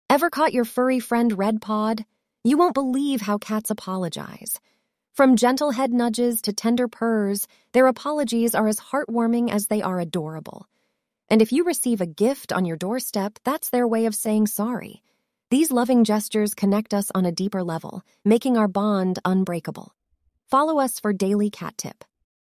How your cat say sorry?
🐾😻 Unbelievable Cat Apologies! Watch these adorable furballs apologize in the sweetest ways, from gentle head nudges to leaving surprise 'gifts' on doorsteps 🎁💕 Don't miss this heartwarming showcase of our furry friends' love and connection! 😍🐾 CatApologies PurrfectBond AdorableApologies FurryFriendsForever 🐱🐾